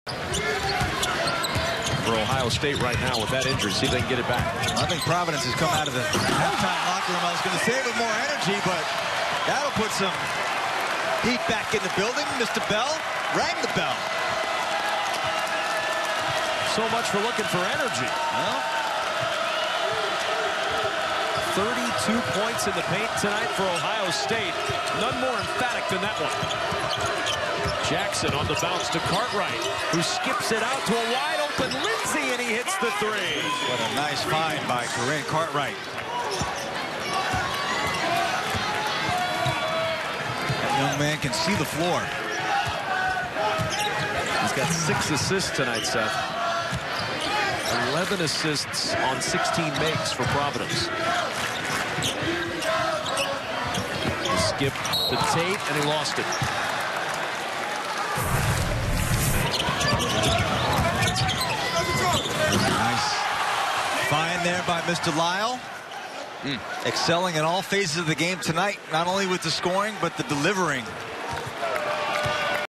Bell Throws Down Alley-Oop vs. Providence
Ohio State's JaQuan Lyle throws up the pass to David Bell for the dunk against Providence.